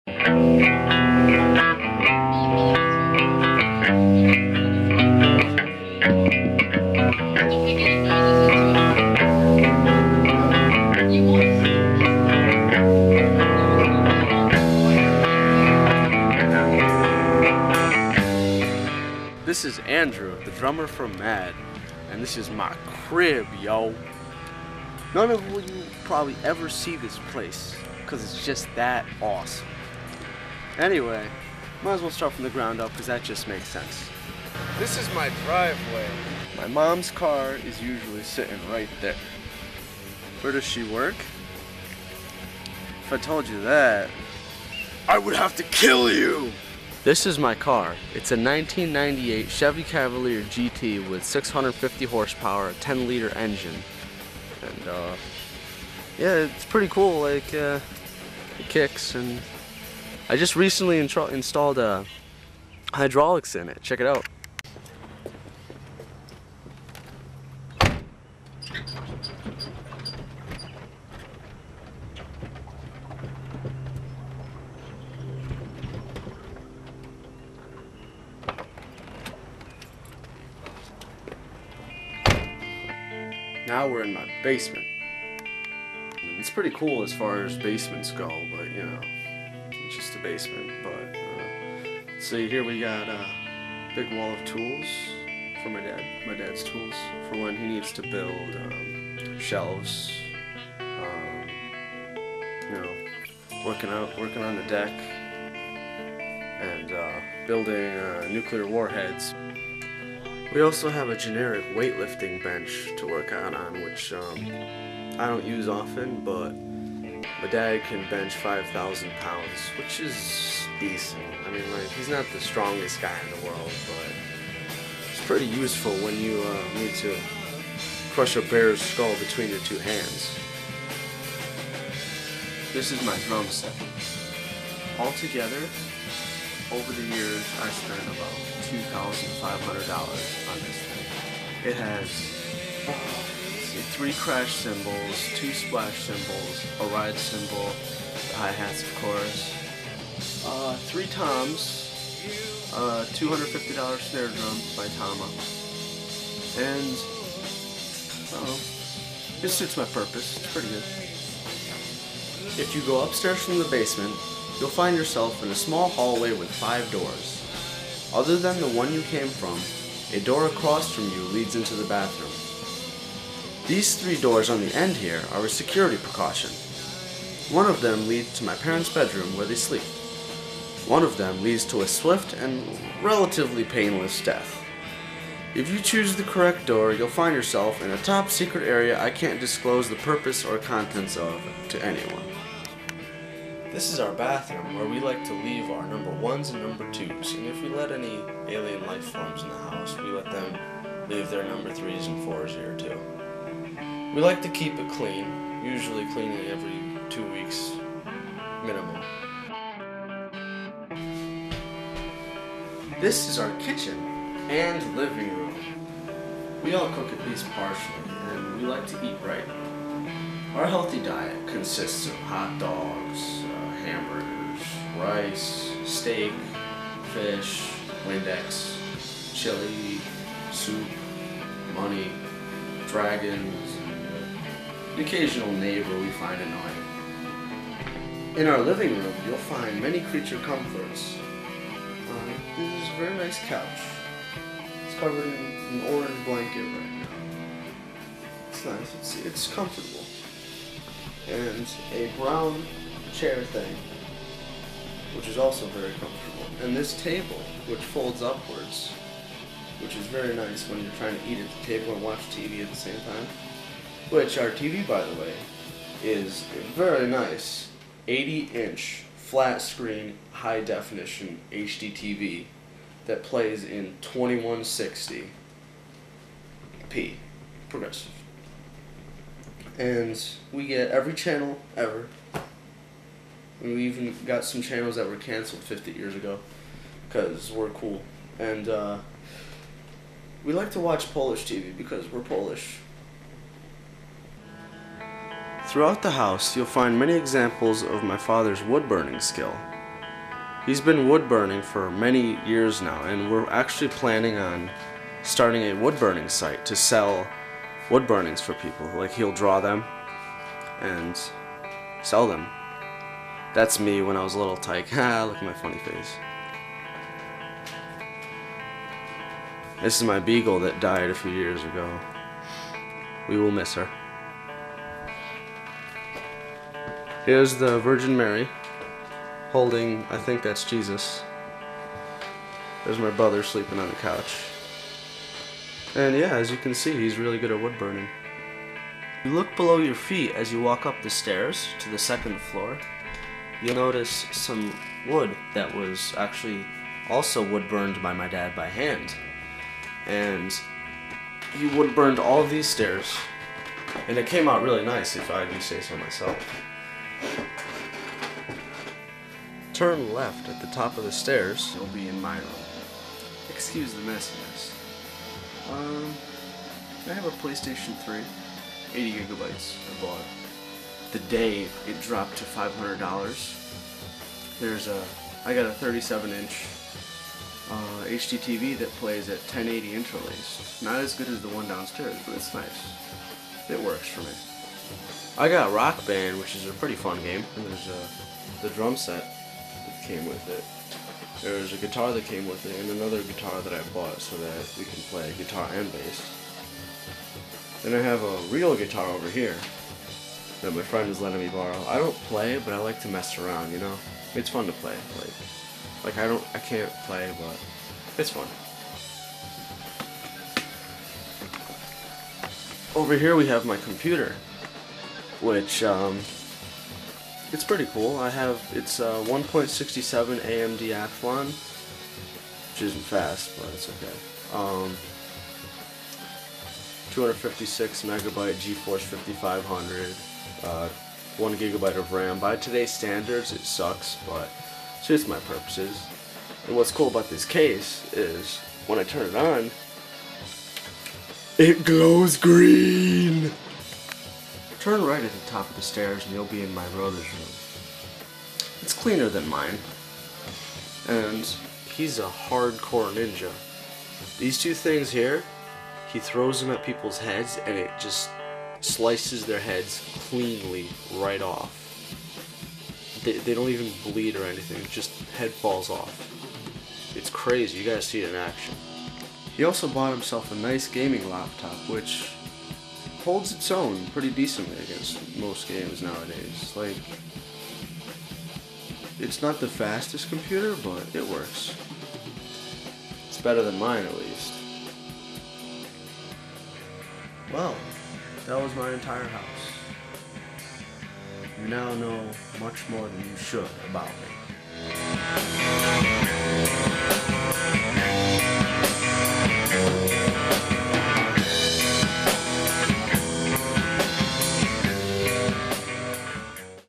M.A.D. Cribz - Andrew's
Cheap, low budget rip off of MTV Cribs. Final project for Video Production II. Took 6 hours to film/edit.